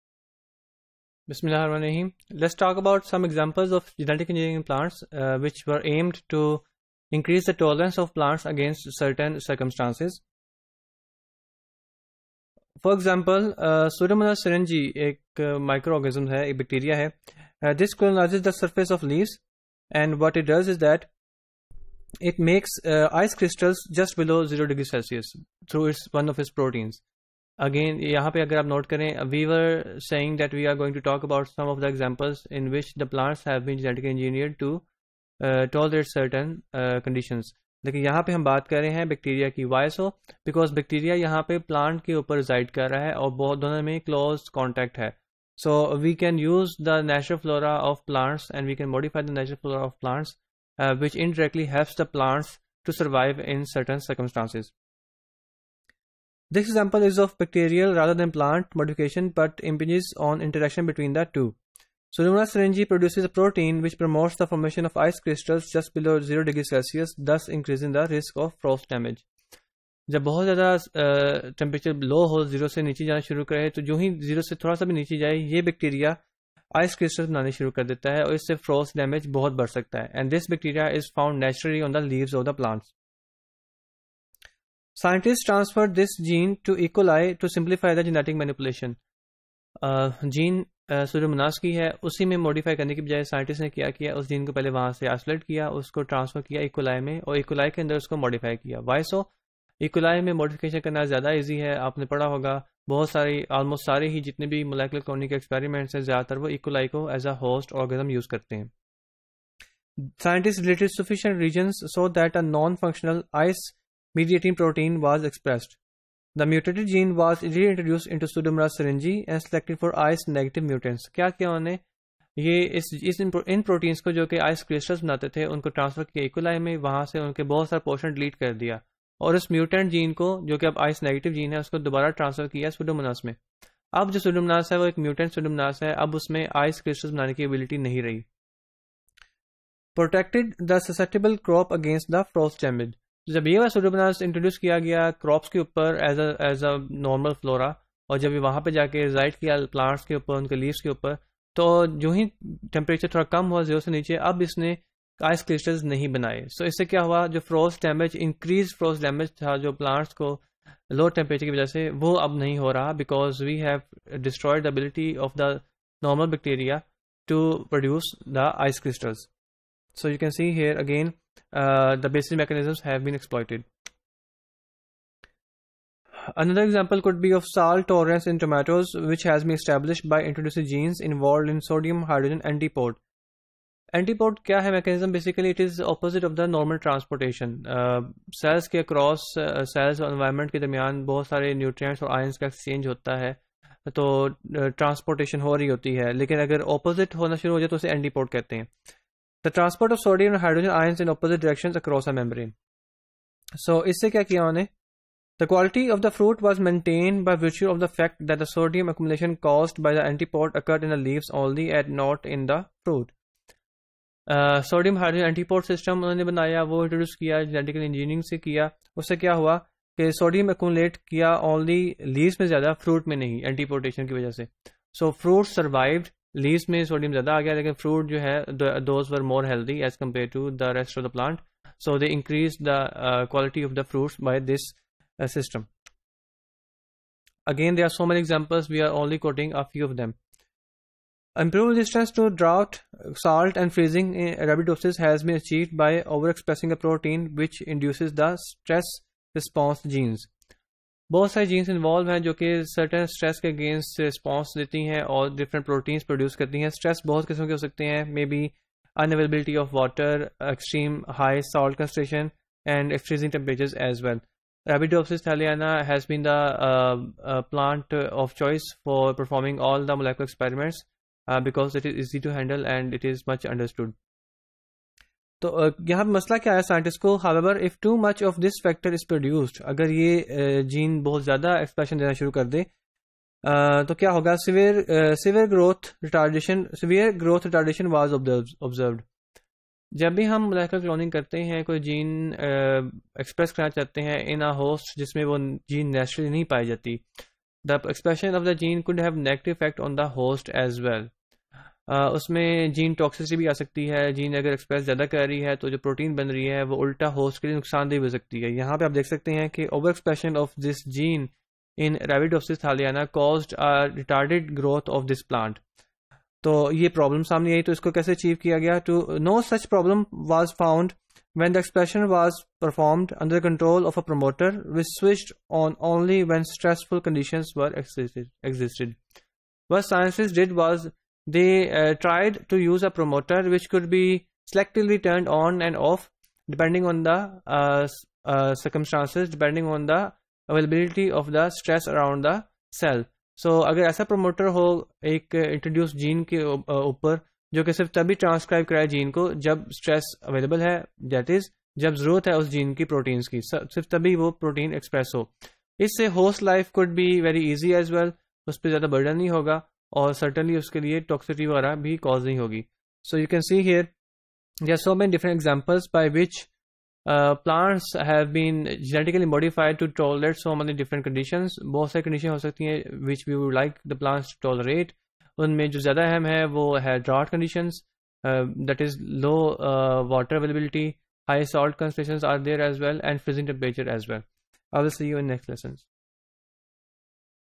BT503_Topic030
BT503 - Environment Biotechnology
30.Selected examples of developments in plant Genetic Engineering: Improved tolerance
Dr. Yasir Rehman